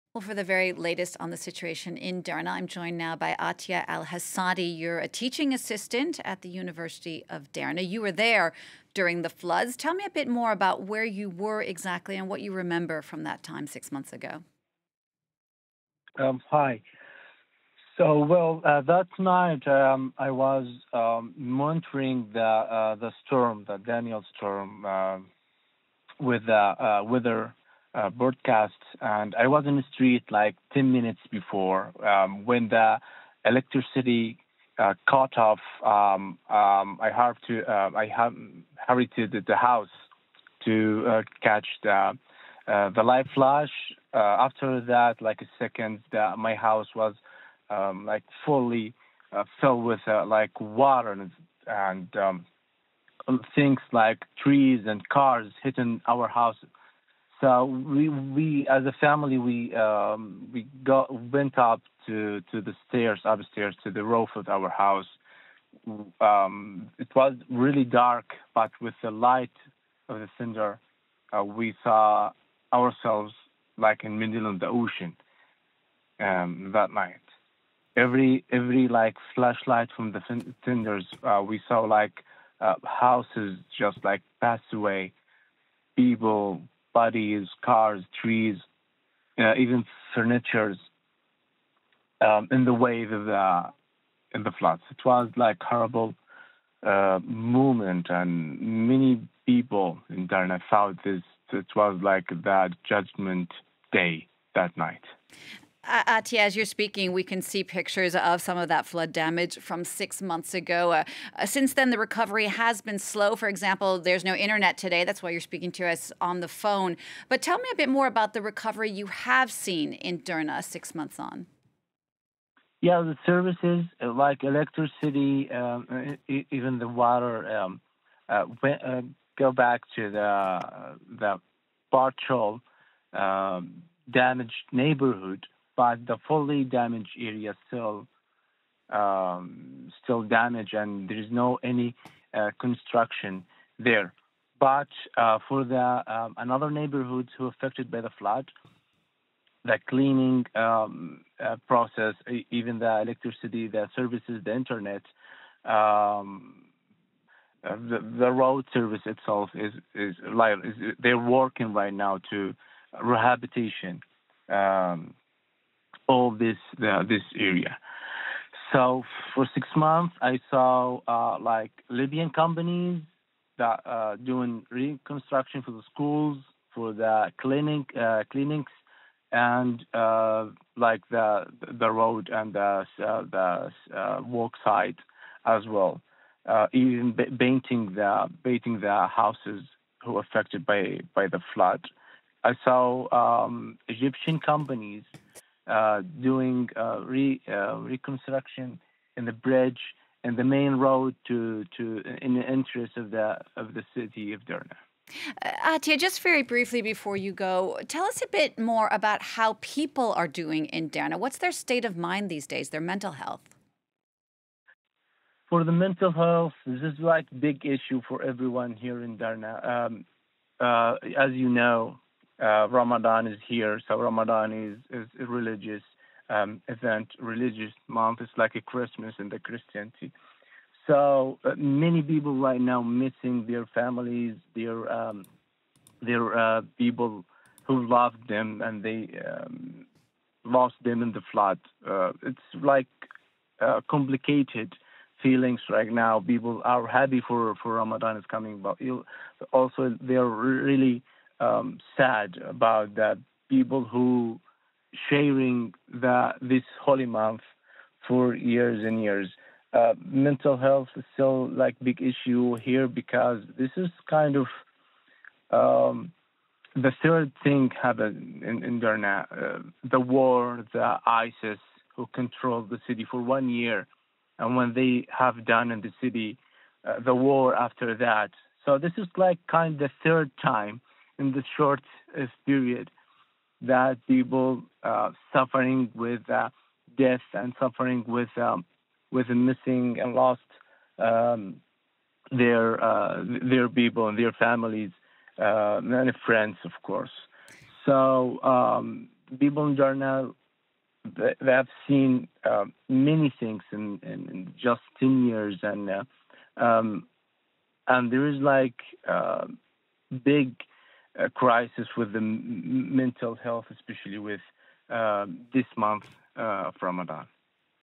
'Mental health a big issue in Derna' six months after floods • FRANCE 24 English
Six months after devastating floods in Libya's Derna killed thousands, people are still struggling to come to terms with the devastation caused and the loss of family members, particularly as the Muslim holy month of Ramadan begins, says Atiyah Alhasadi, Astrophysicist, S.T.E.M. Blogger, Chairman of Roaya for Astronomy and Space Applications Foundation and Teaching Assistant in the Physics Department at Derna University. He was a witness to the deadly floods.